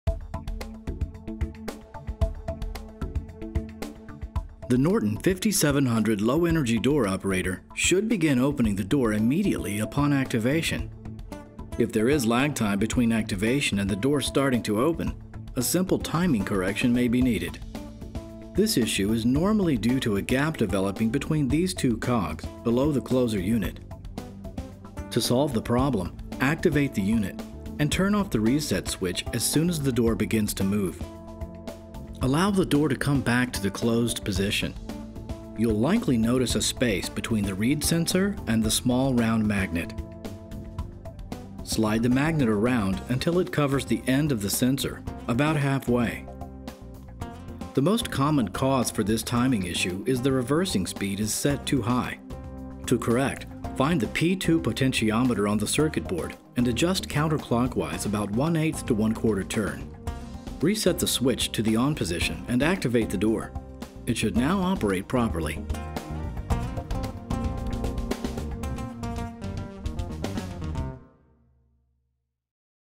5700 Operator: How to Correct Timing - Technical Product Support
Learn how to correct the timing of a 5700 Operator. Watch as this Norton Rixson Door Controls expert fixes the timing.